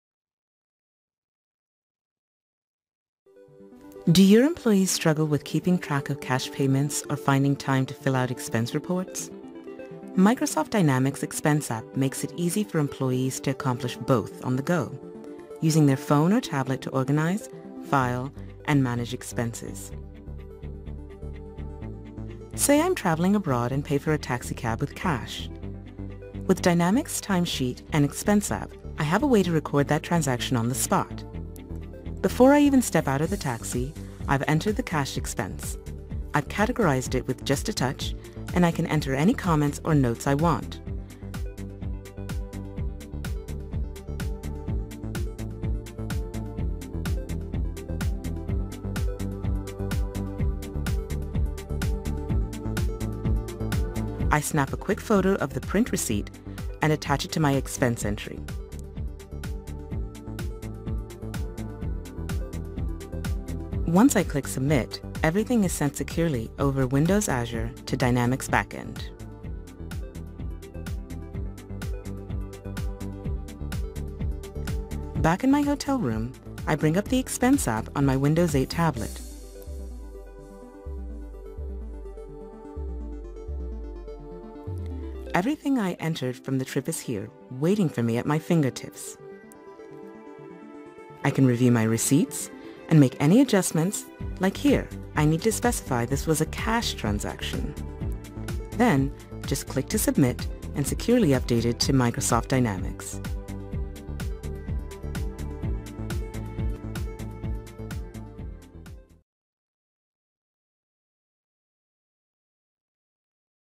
Mobile Expense App | Dynamics 365 AX | Western Computer
Another great video provided by Microsoft. See how the Microsoft Dynamics Expense app helps you record and categorize cash payments and file expense reports right from your smartphone or tablet. No more keeping track of miscellaneous paper receipts --digitalize print receipts on the spot with a quick photo, attach it to your expense entry, and securely submit over Windows Azure to the Dynamics back end.